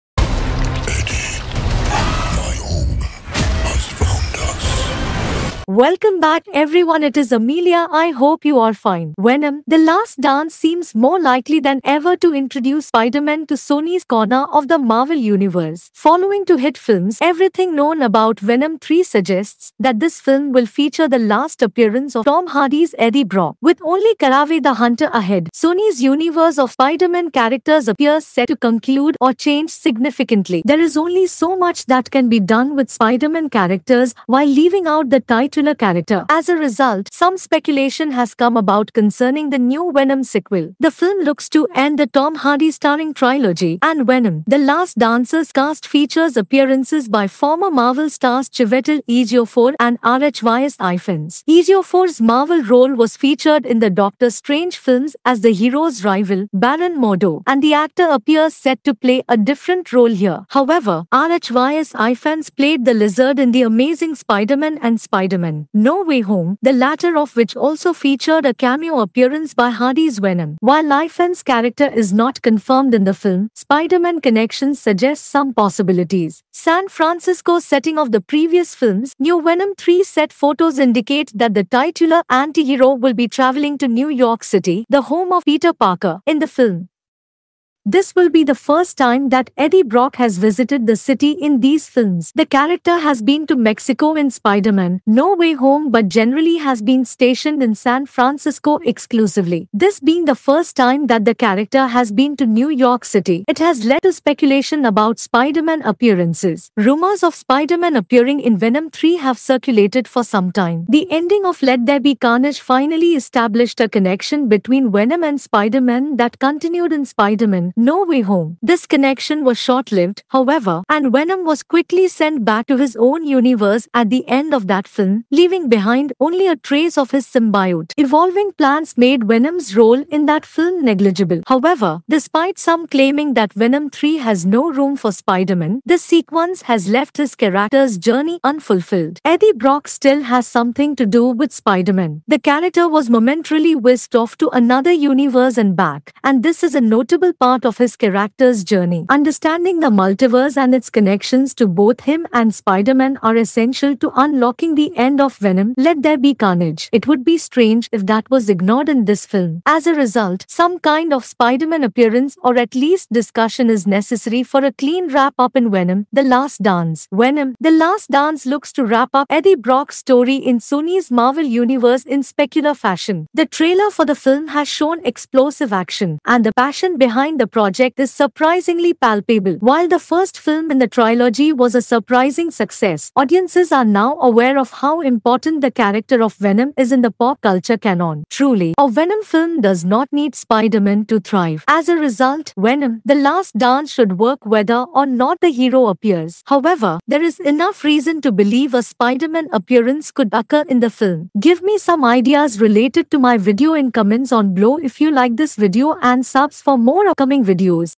Venom 3: Spider-Man's Arrival in Sony’s Marvel Universe Just Got Way More Likely
With Venom 3: The Last Dance on the horizon, the introduction of Spider-Man into Sony’s Marvel Universe seems more possible than ever! In this video, we explore the clues, fan theories, and latest news that point to Spidey’s long-awaited crossover with Venom. Could this be the moment fans have been waiting for? Dive into all the exciting possibilities.